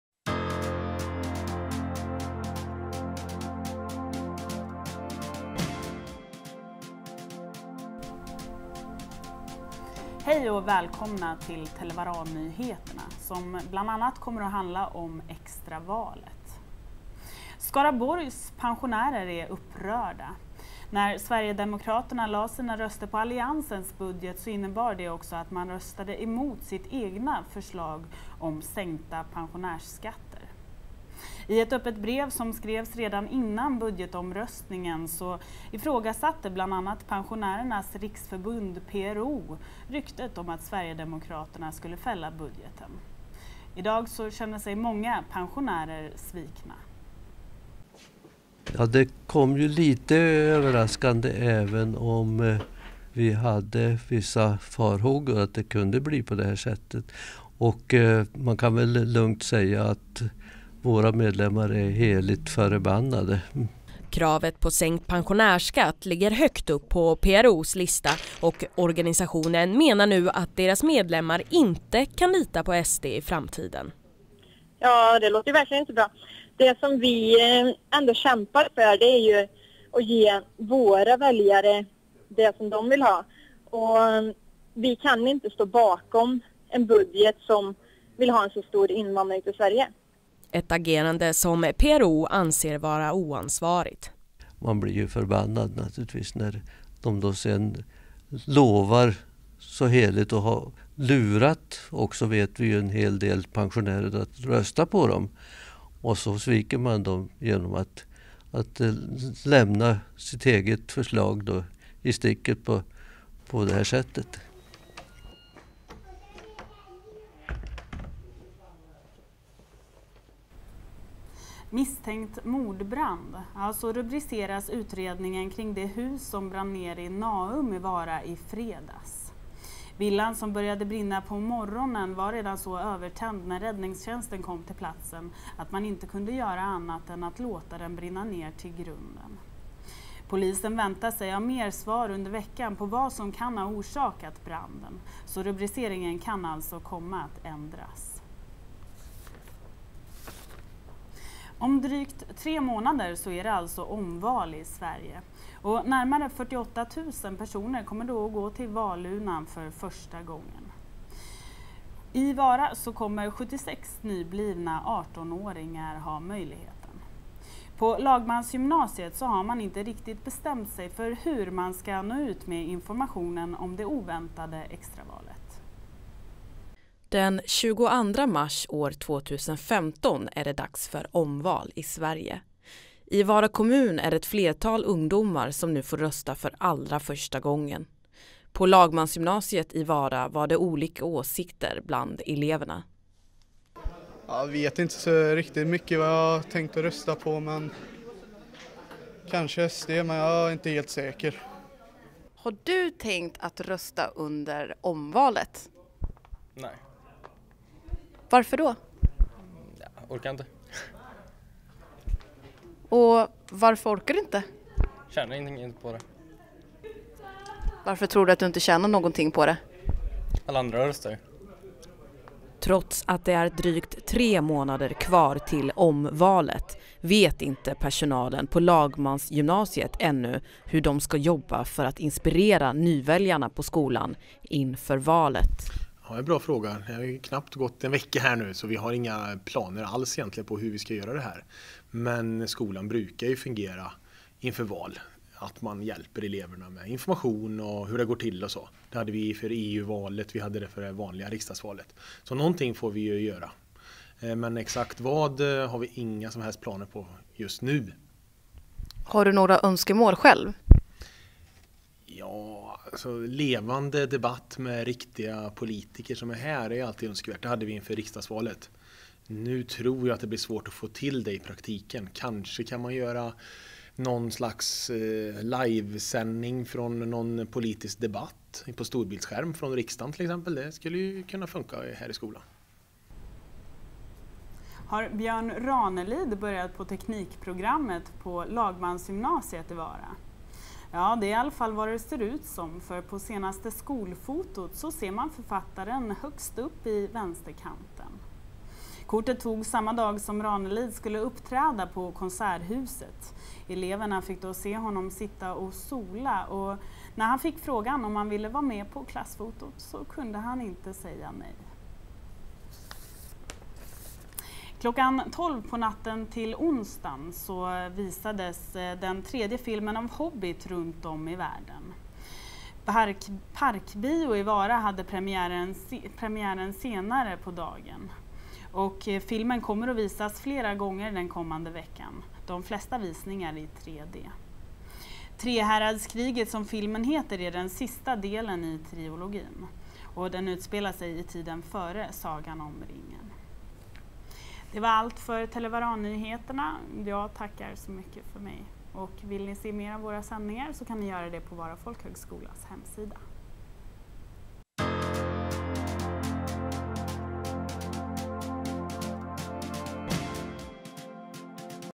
Nyheter med Televaran 11/12 del 2
Nyheter från Televaran med fokus på det kommande extra valet.
Produceras av journalisteleverna på Vara Folkhögskola.